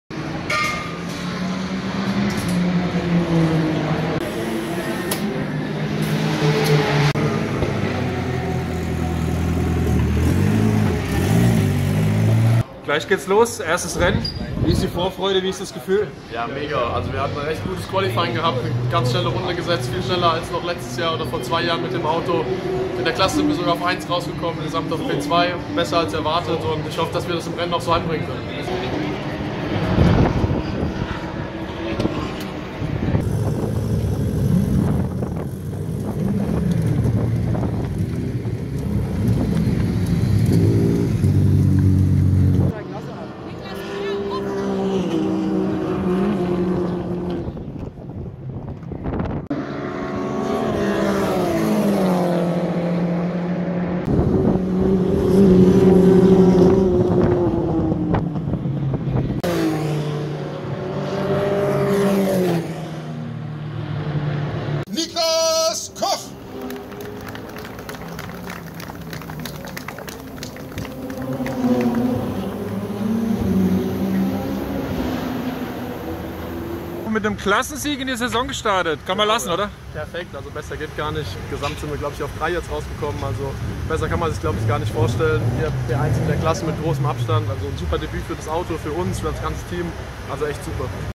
Starker Saisonstart für Rennfahrer Niklas Koch in Hockenheim
Niklas Koch ist mit einem Klassensieg in die Saison gestartet. Der Autorennfahrer aus Riedstadt begeistert mit seinem grünen Mini in der Tourenwagenserie DMV BMW-Challenge. Mehr dazu